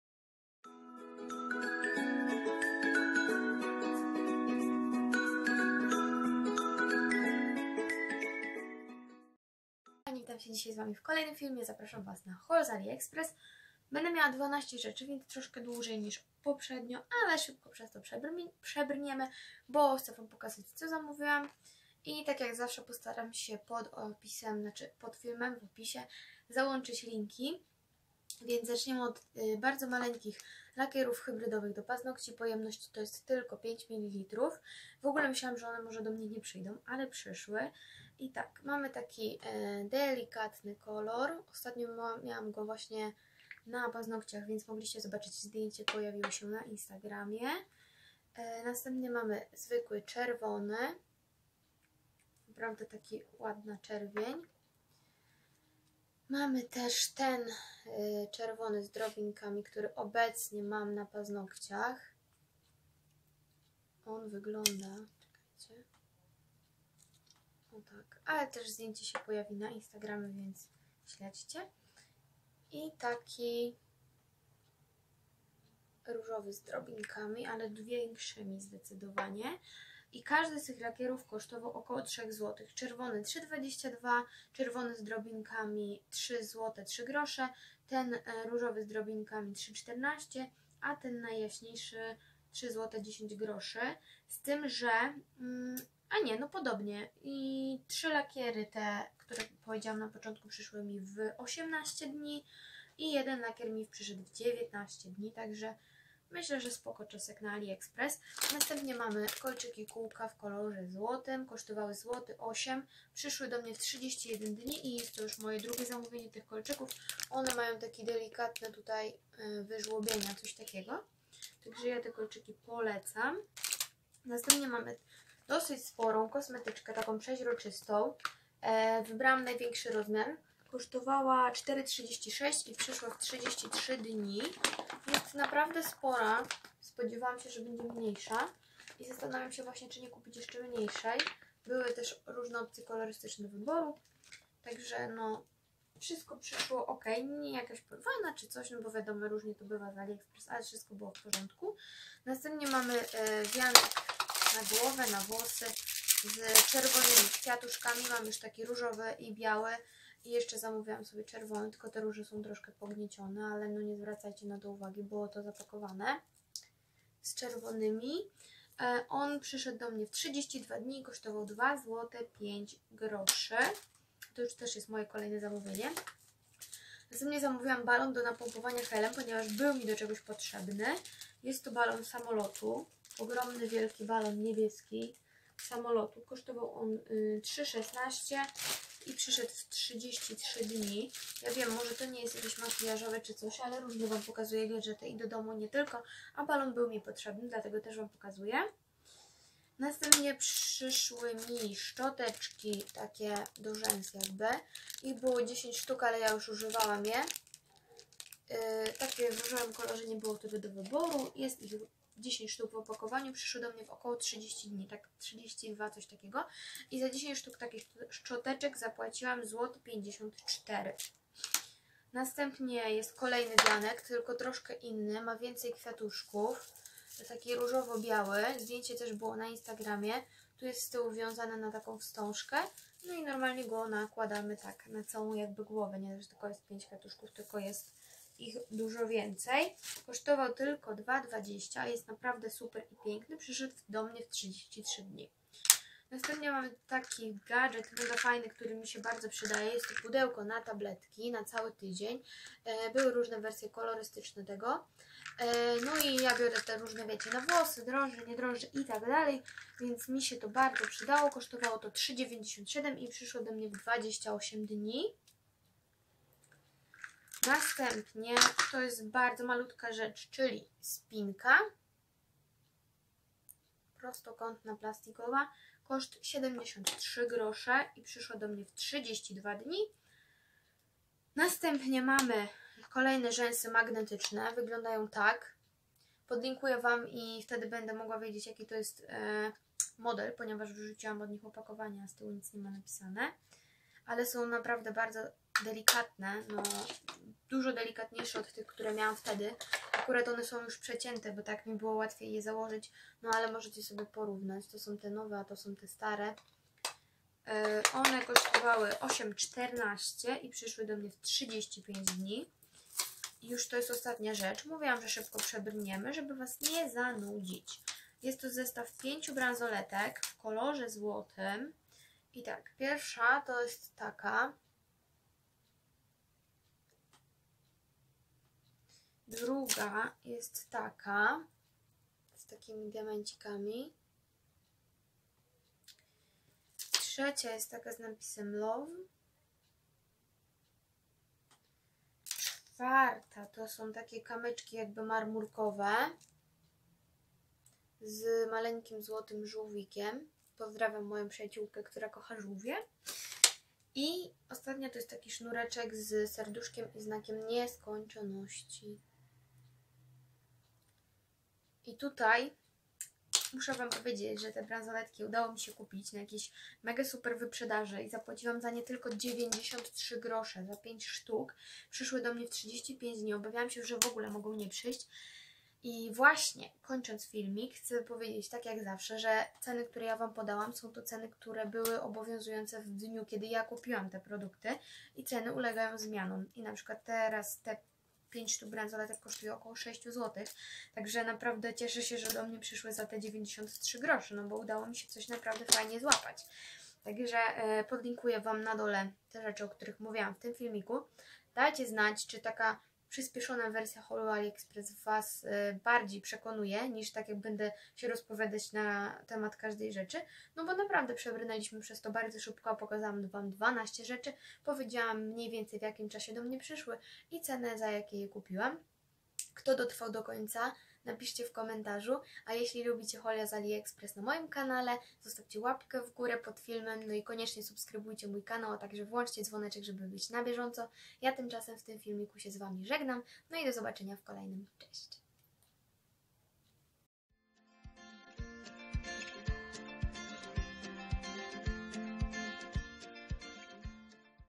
💟HAUL ALIEXPRESS #8💟 | Wianki, kolczyki, ozdoby, gadżety.. 🤩
Hej! Zapraszam na kolejny haul z aliexpress ♡
W tym filmiku 12 rzeczy, którymi chcę się z Wami podzielić 😊 Jeśli jesteście zainteresowani jakąś z nich to na samym dole opisu znajdziecie linki do aukcji tych rzeczy 🥰

Jeśli filmik Ci się podobał i lubisz haule zakupowe zostaw pod filmem łapkę w górę 👍🙂

Zapraszam na instagrama kanału: followyourdreams_yt 🤗